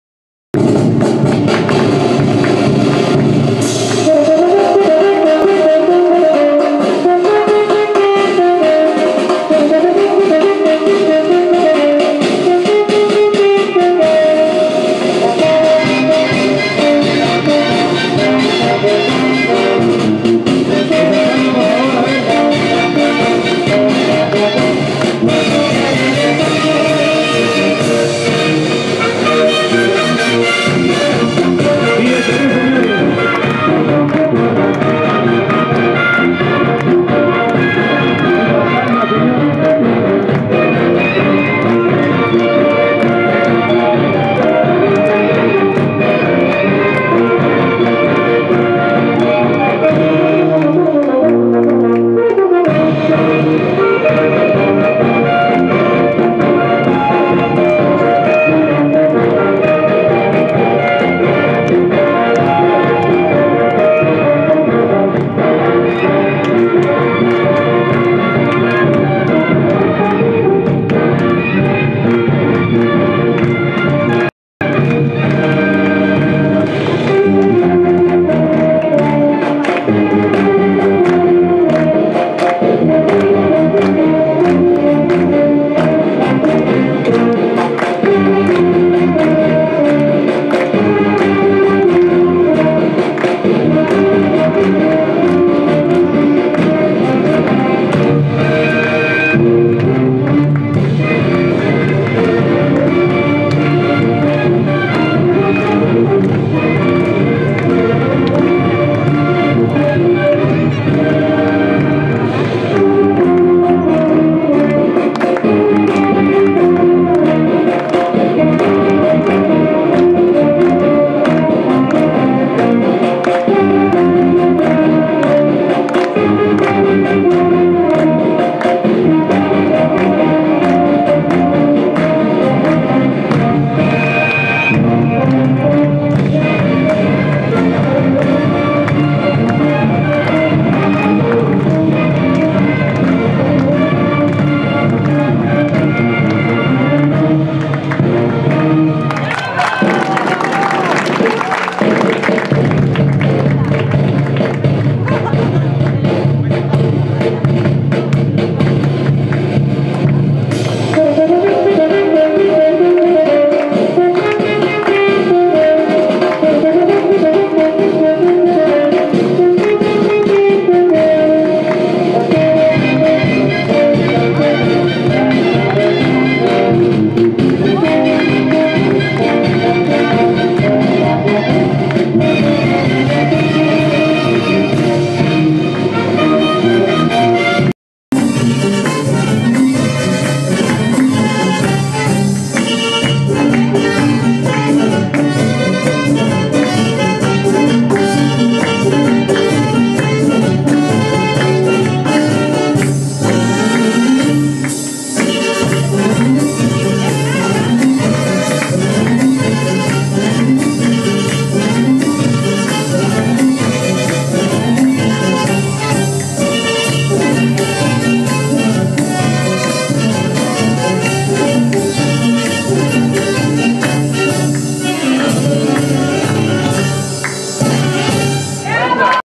milagros2015dance
ペルーの民族舞踊、マリネラが披露されました。